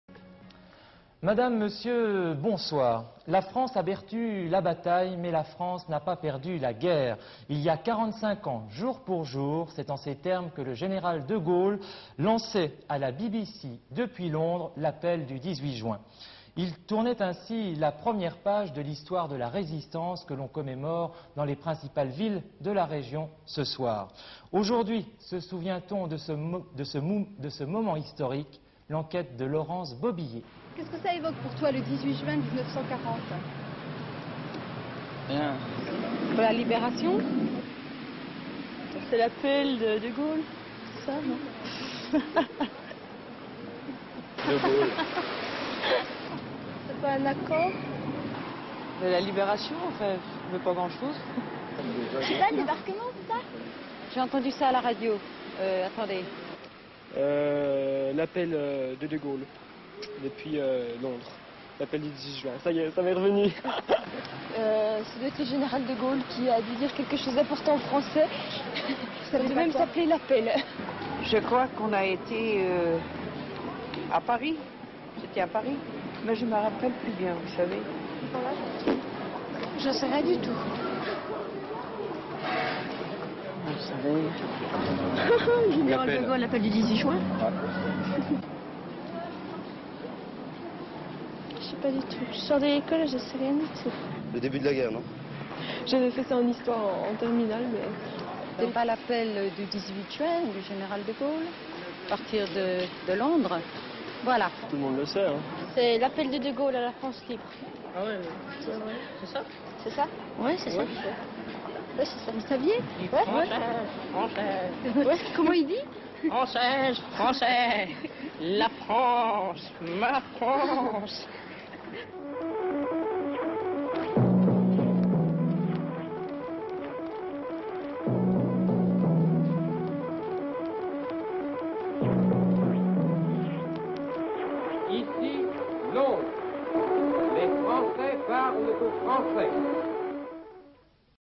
Qu'est-ce que l'appel du 18 juin ? | Archive INA
18 juin 1985
A l'occasion du 45ème anniversaire de l'appel du 18 juin, les passants sont interrogés sur cet évènement, dans les rues piétonnes de BESANÇON.Un très petit nombre des personnes interrogées ont pu dire que l'appel du général de GAULLE a été lancé un 18 juin à la BBC, depuis LONDRES.Un jeune collégien en fait une imitation. Images d'archive INA